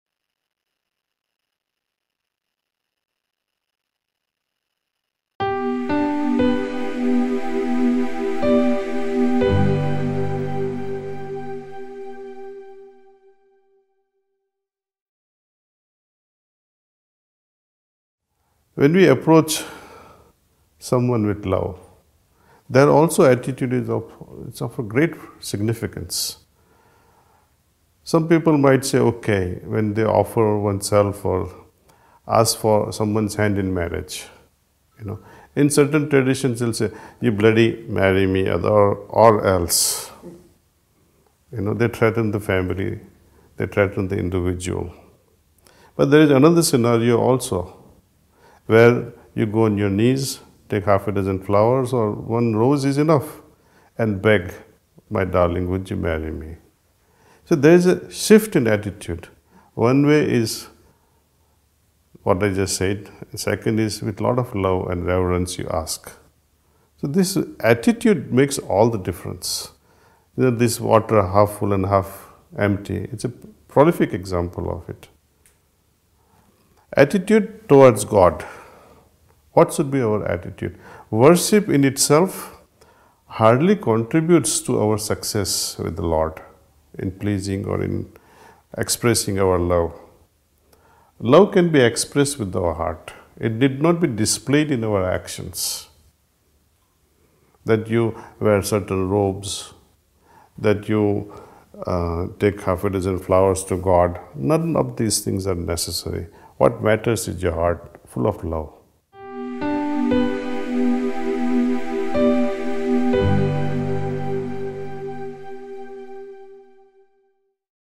Yoga For Modern Life by Daaji | Ep 5 : Attitude Full Of Love | Spirituality in Modern Life
These spiritual golden nuggets are delivered by Daaji, which is a prefect blend of ancient wisdom, meeting the demands of a modern life.

Episode 5 : Yoga for Modern Life by Daaji : Ep 5 - 'Attitude Full Of Love'
What is more important in our relationship with our Creator? Worshiping Him or Attitude full of Love?

In the 5th Episode of series 'Yoga for Modern Life', Daaji, Kamlesh D Patel explains how should our attitude be towards the God.

Listen to this episode and please do share the golden nuggets with your family and friends.

Toll free number (India): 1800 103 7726
Toll free number (International): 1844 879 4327